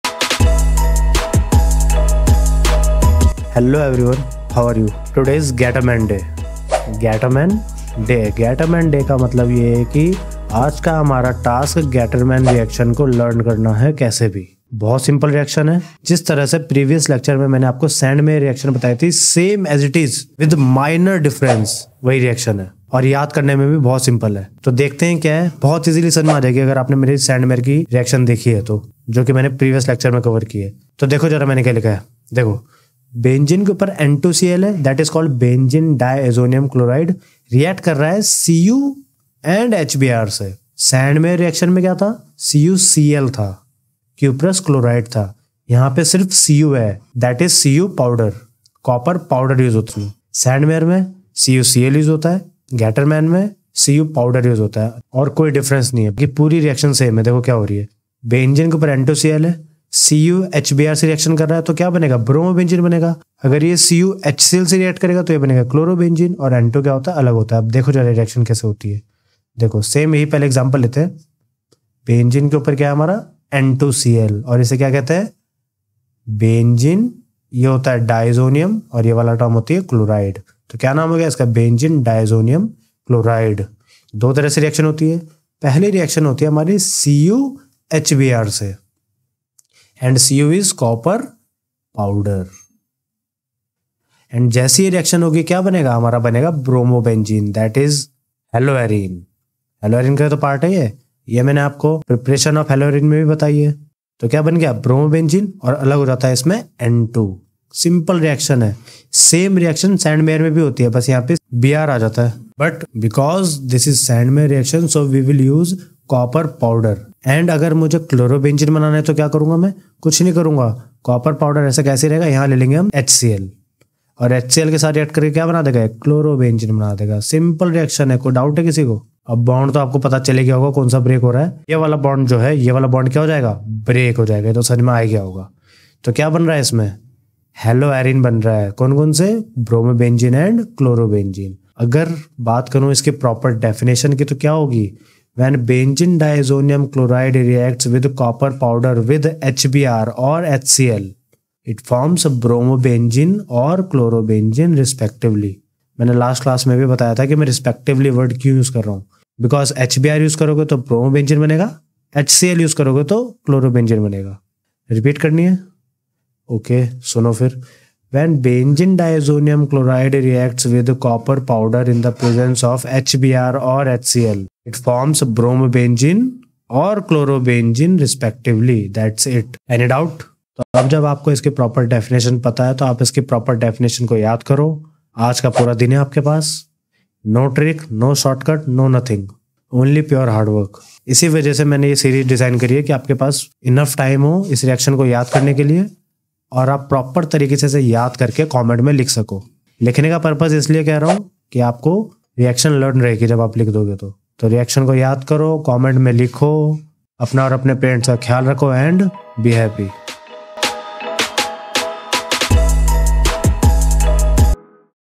Gattermann Reaction | 30 Days 30 Name Reactions Class 12 | Organic Chemistry | NCERT | CBSE
This is 6th Day of 30 Days 30 Name Reactions In Which We Will Learn Gattermann Reaction. So Join the 30dayschallenge of 30days30namereactions & Make Sure You Put The Definition in The Comment Section Today.
30 Days 30 Name Reactions Class 12:
Topics Covered
00:00 Gattermann Reaction
03:00 Free Notes & Series

If You Are New Here Comment " learntodefine " Welcome To The TGFamily❤.